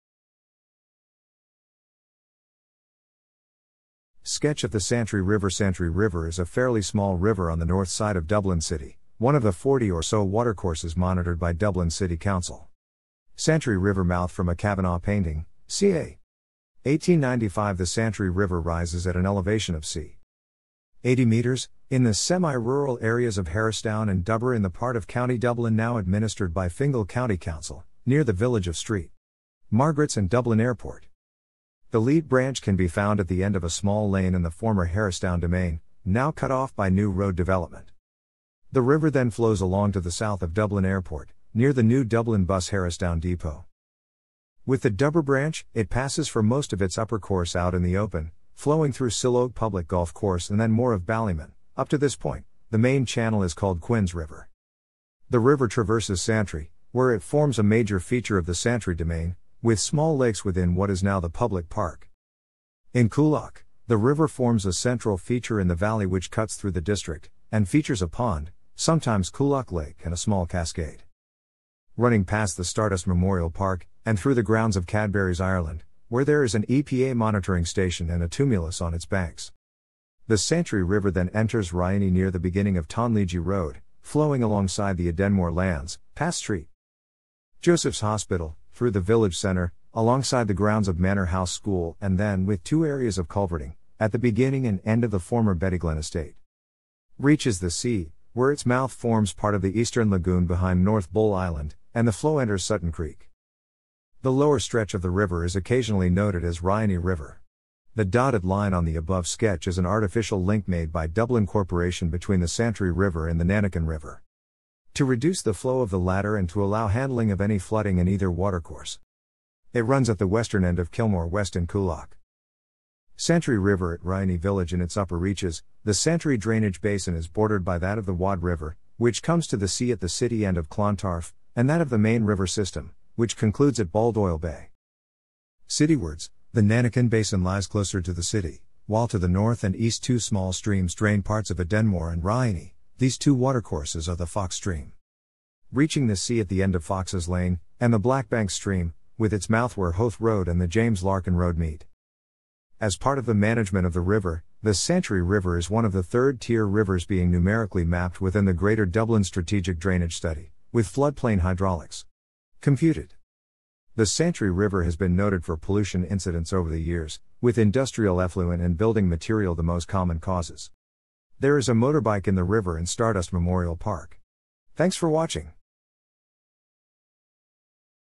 Santry River
Rivers_of_Dublin_(city)
Sketch of the Santry River Santry River （Irish: Abhainn na Culoige） （formerly Skillings Glas） is a fairly small river on the north side of Dublin city, one of the forty or so watercourses monitored by Dublin City Council.
Santry River mouth from a Kavanagh painting, ca.
1895 The Santry River rises at an elevation of c.
80m, in the semi-rural areas of Harristown and Dubber in the part of County Dublin now administered by Fingal County Council, near the village of St.
Margaret's and Dublin Airport.
The lead branch can be found at the end of a small lane in the former Harristown Demesne, now cut off by new road development.
The river then flows along to the south of Dublin Airport （from which some tributary streams enter it）, near the new Dublin Bus Harristown depot.
With the Dubber branch, it passes for most of its upper course out in the open, flowing through Sillogue Public Golf Course and then more of Ballymun; up to this point, the main channel is called Quinn's River.
The river traverses Santry, where it forms a major feature of the Santry Demesne, with small lakes within what is now the public park.
In Coolock, the river forms a central feature in the valley which cuts through the district, and features a pond, sometimes Coolock Lake, and a small cascade,
running past the Stardust Memorial Park, and through the grounds of Cadbury's Ireland, where there is an EPA monitoring station and a tumulus on its banks.
The Santry River then enters Raheny near the beginning of Tonlegee Road, flowing alongside the Edenmore lands, past St.
Joseph's Hospital, through the village centre, alongside the grounds of Manor House School, and then, with two areas of culverting, at the beginning and end of the former Bettyglen Estate,
reaches the sea, where its mouth forms part of the eastern "lagoon" behind North Bull Island, and the flow enters Sutton Cree...